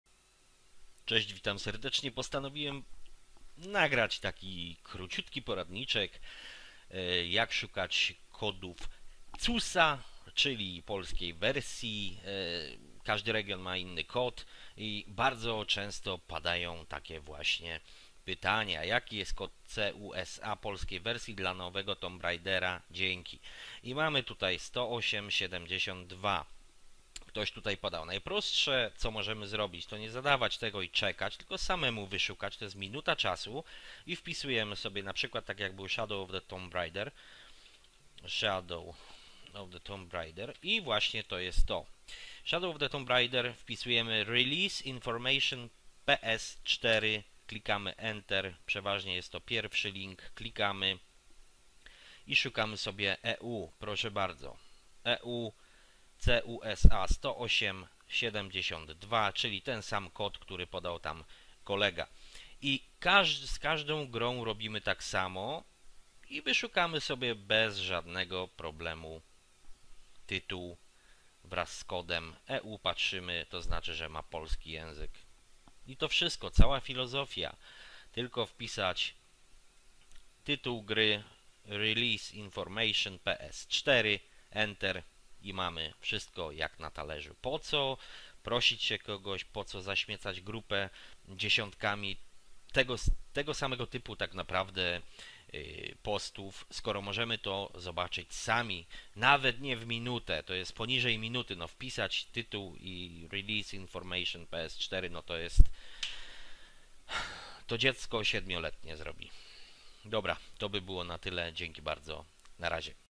Jak wyszukać kod CUSA dla PS4?
Wiele osób poszukuje kod CUSA, w filmie prosta odpowiedź i zajmuje to minutę czasu.